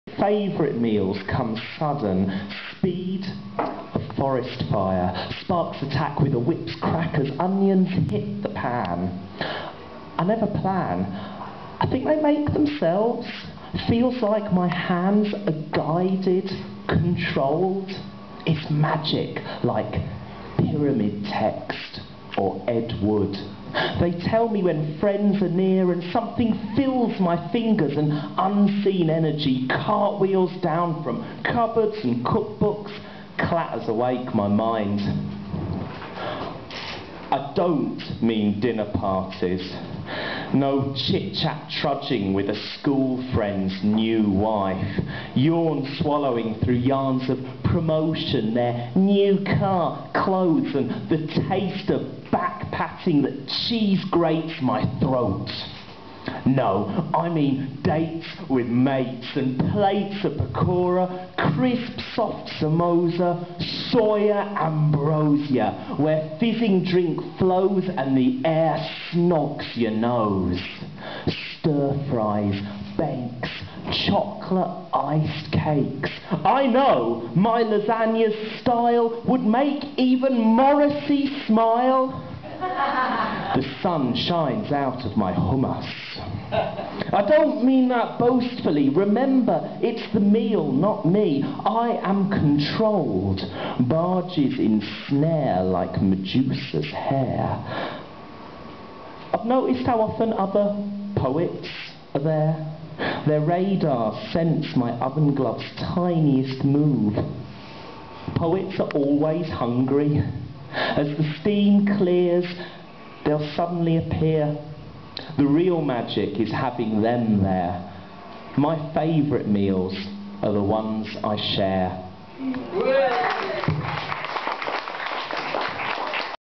Dominic Berry 'My Favourite Meals'
from a Write Out Loud night in Bolton. Poem taken from the book Tomorrow I Will Go Dancing